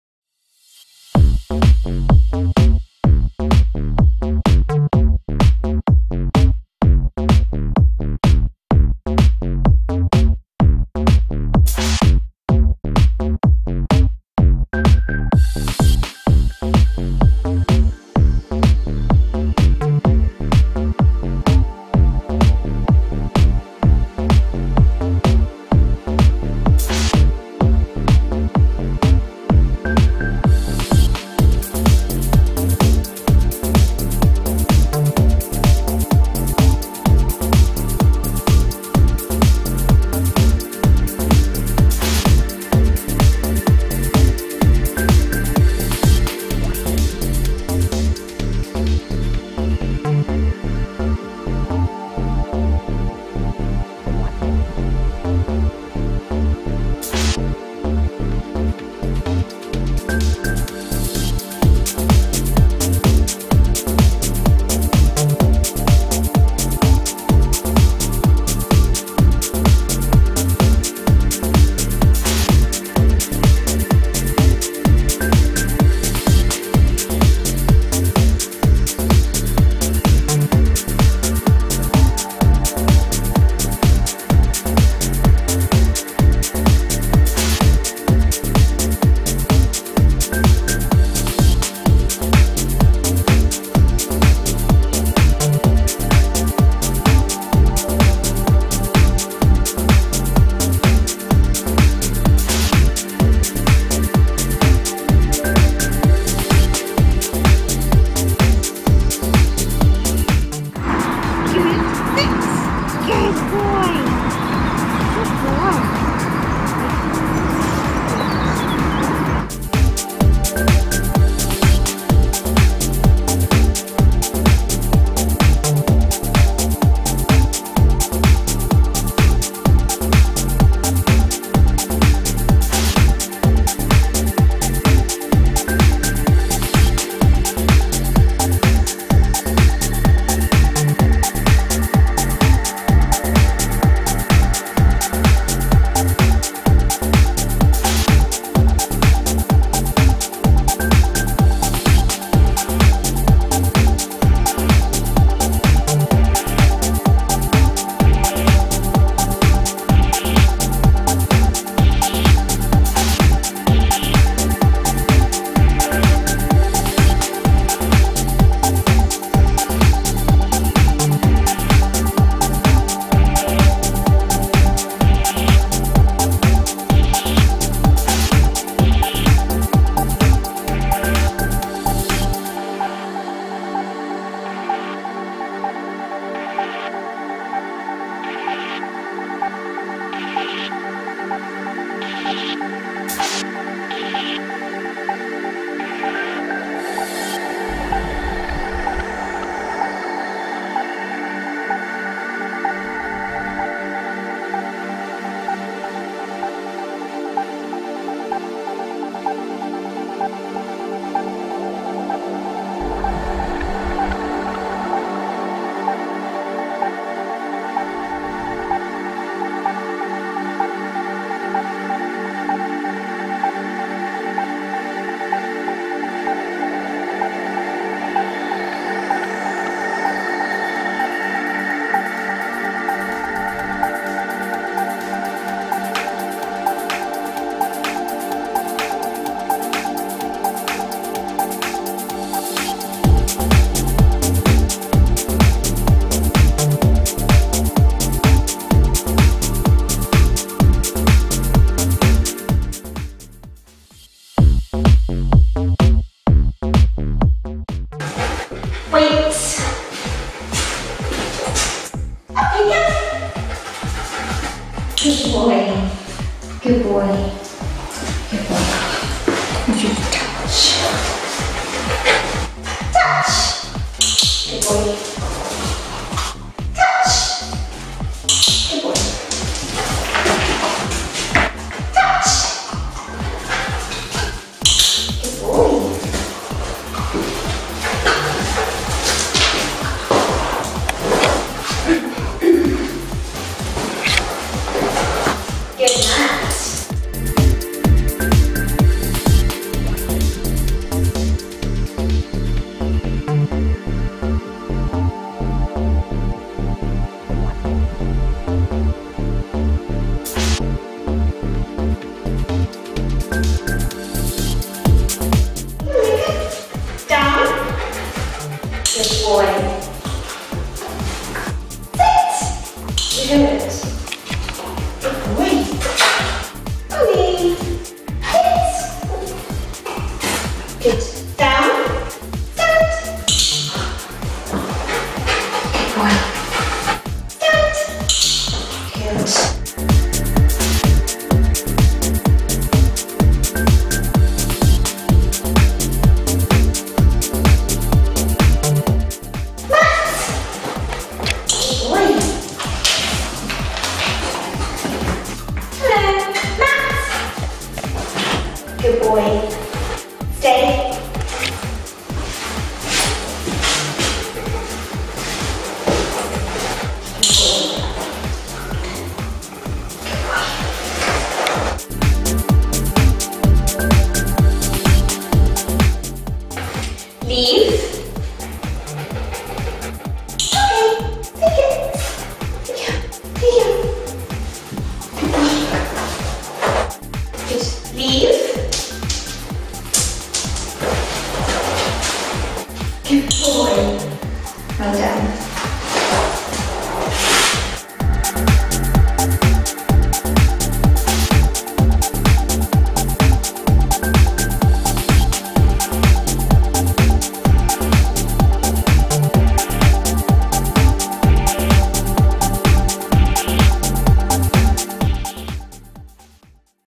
"Domino" (adopted)
presents doodleDogs' "Domino" . A MERLE 2 coloured MALE Australian Service Dog (ASD) Australian Labradoodle. As always our lines are extensively screened and vet checked. The adoption cost includes spay/neuter, microchip (& transfer to you), flea/worm treatment, 4 weeks insurance to UK families, age-appropriate vaccines incuding the extra vacines (kennel cough and corona virus).Partly trained ideal for families who prefer not to have a pre-vaccine puppy or simply want the ease and convienence of a puppy parlty trained